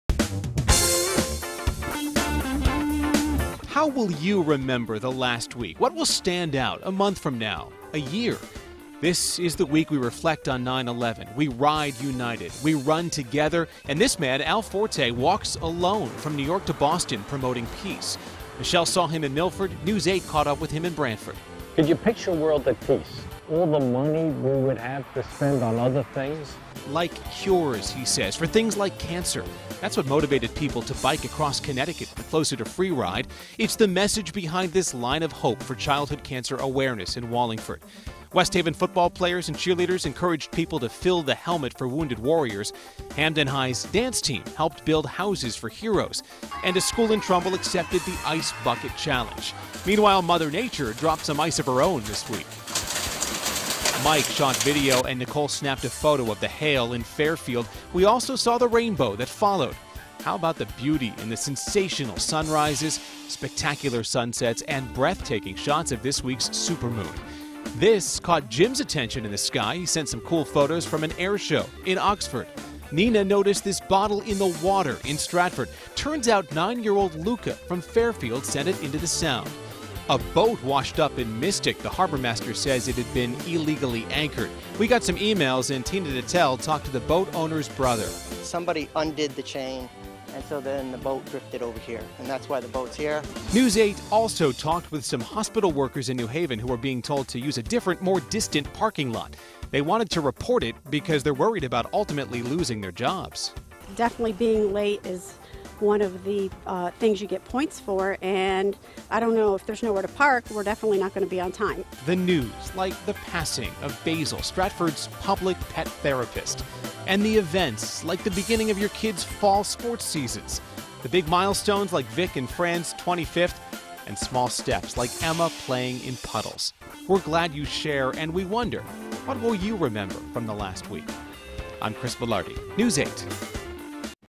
Report It Recap
(WTNH) -- News 8's Chris Velardi looks at the past week with your Report It photos, videos and story suggestions.